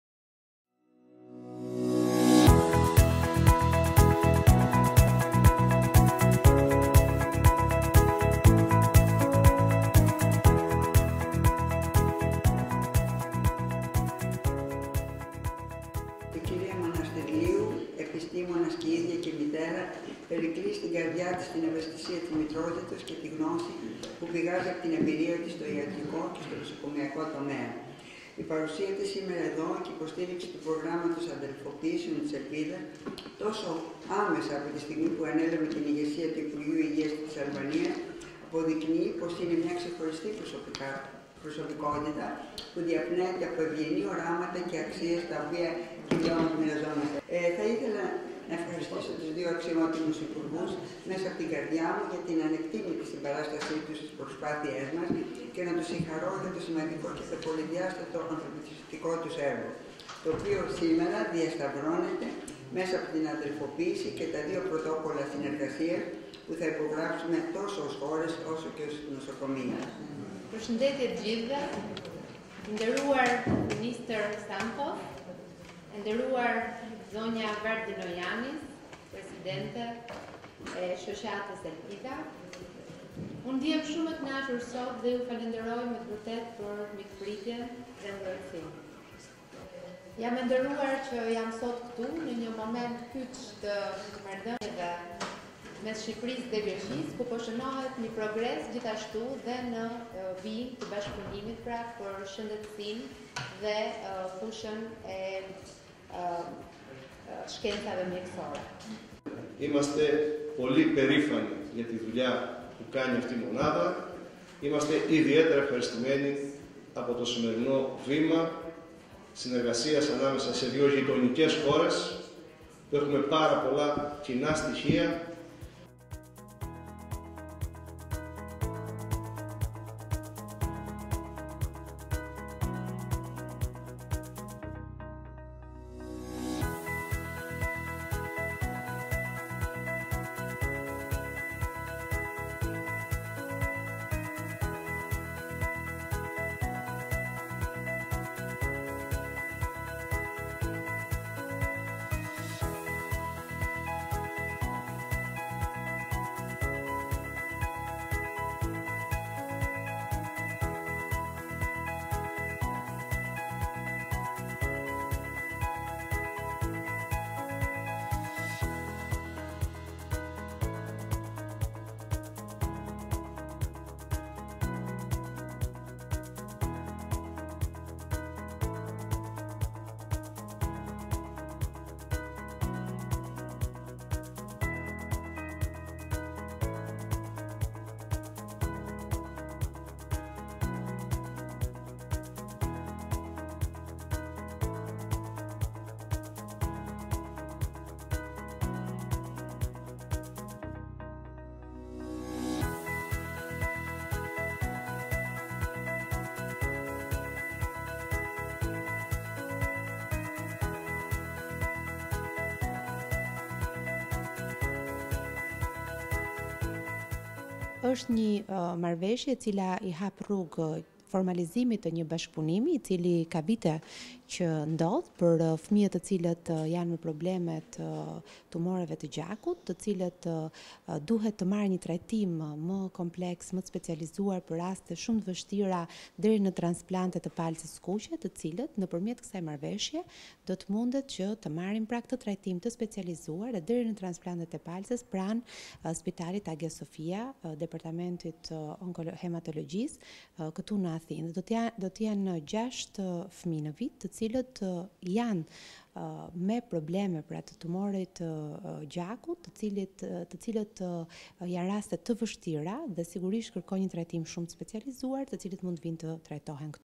QSUT binjakëzohet me spitalin “Elpida” të Mariana Vardinoyanis, për fëmijtë me leucemi dhe tumore
Midis Greqisë dhe Shqipërisë është krijuar një urë bashkëpunimi për fëmijtë shqiptarë që vuajnë nga kanceri.
Memorandumi parashikon trajtimin e fëmijëve shqiptarë të diagnostikuar me leuçemi dhe tumore malinje, përmes binjakëzimit të spitalit Agia Sofia dhe Qendrës Spitalore Universitare Tiranë “Nënë Tereza”.
Marianna V.Vardinoyanis, Presidente e Fondacionit “Elpida”:
“Me këtë marrëveshje hapin një fazë të re bashkëpunimi mes Greqisë dhe Shqipërisë, duke përcjellë mesazhin te familjet këtej dhe andej kufirit se ka një shpresë për trajtimin e fëmijëve me kancer që kanë nevojë për transpalnt të palcës”.